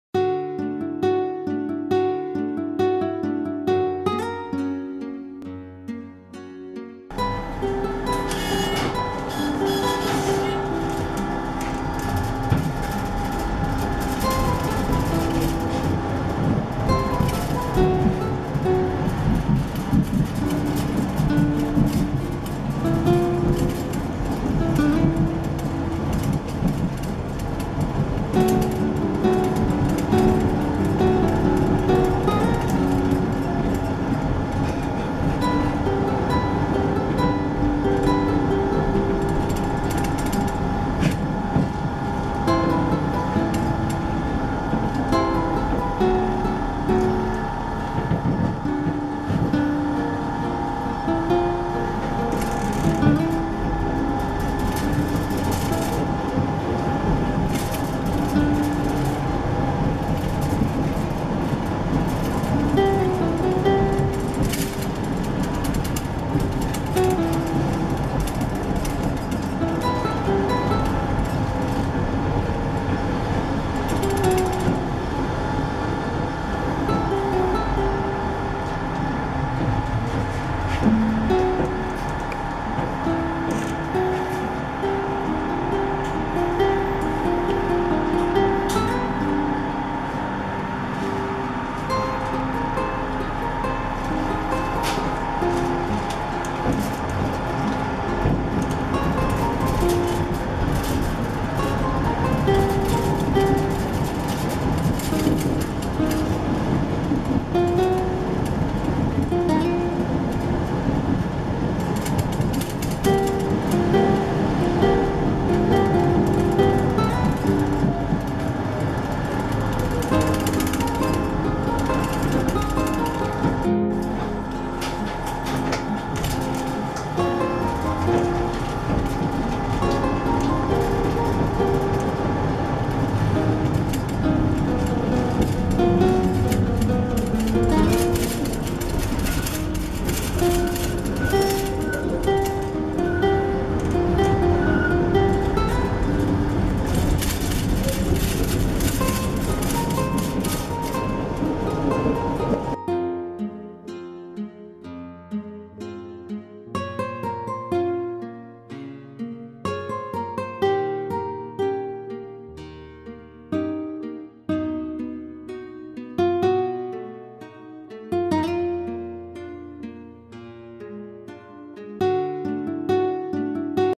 2020 08 31 Warszawa Ulica Marszałkowska cz 1 widok z tramwaju po 12 43
Ulica Marszałkowska / Marszałkowska Street / Rue de Marszałkowska
Plac Zbawiciela / Savior Square / Place du Sauveur
widok z tramwaju / view from the tram / vue depuis le tram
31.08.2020 po / after / après 12.43